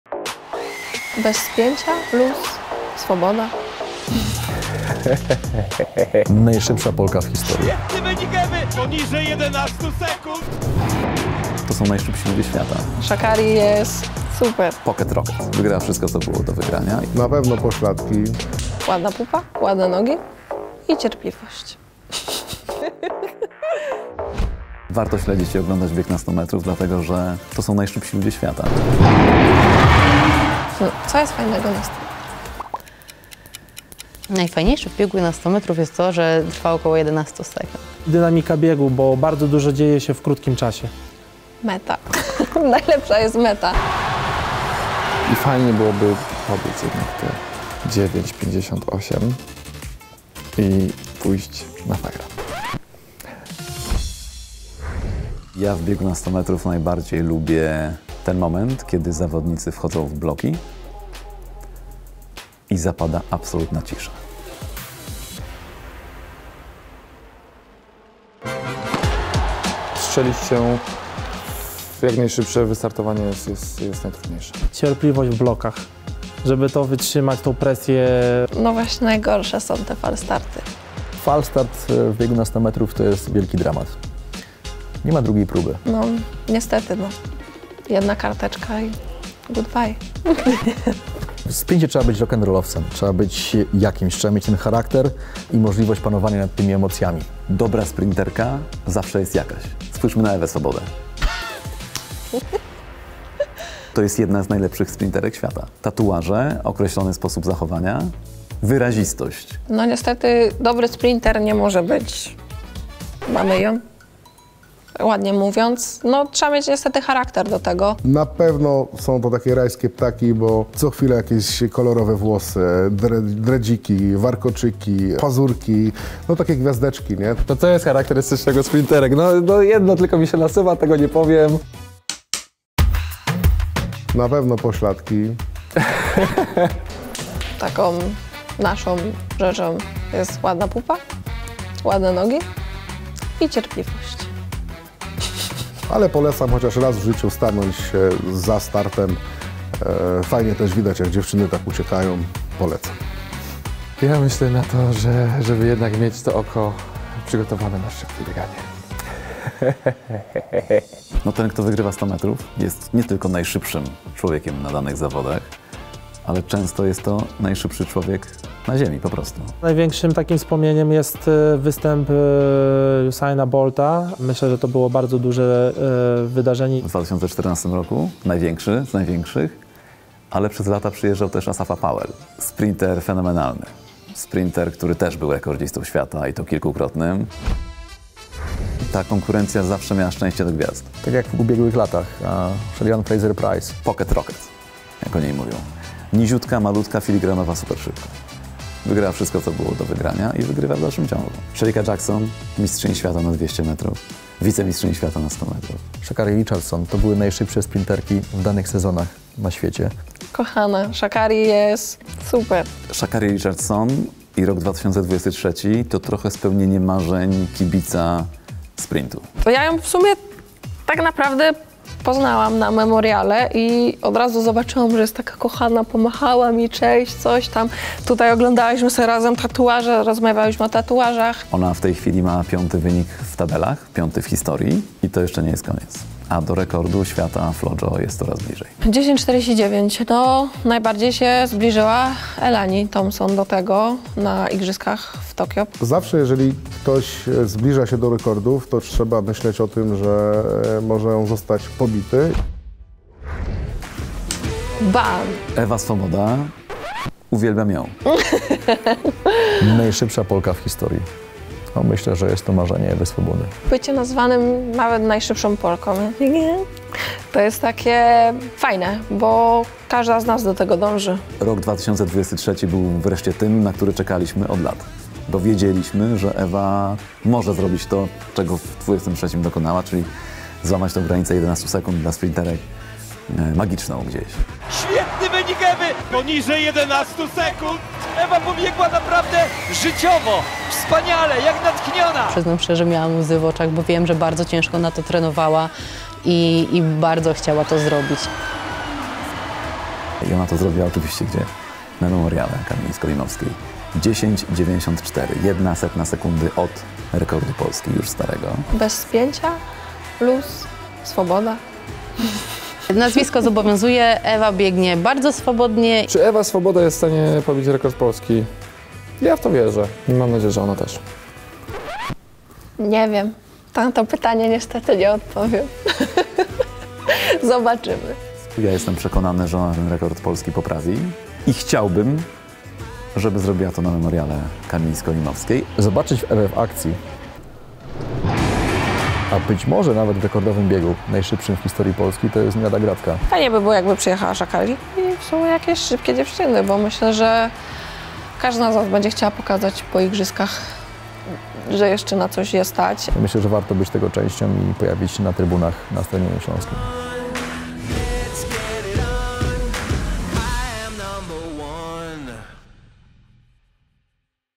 "ŁADNA PUPA, ŁADNE NOGI I CIERPLIWOŚĆ" – najlepsze sprinterki świata to mają!
Podczas ostatniej edycji Silesia Memoriału Kamili Skolimowskiej pobiła rekord życiowy, a ostatnio zdobyła srebrny medal w biegu na 60 m podczas Halowych Mistrzostw Świata. W kolejnym odcinku serii "Lekka Gadka" Ewa Swoboda oraz jej reprezentacyjni przyjaciele opowiedzą o biegu na 100 metrów oraz gwiazdach takich jak Sha'Carri Richardson, Shericka Jackson, Shelly-Ann Fraser-Pryce, czy Usain Bolt i Asafa Powell.

✅ Upewnij się, że zostawiłeś follow po więcej takich materiałów!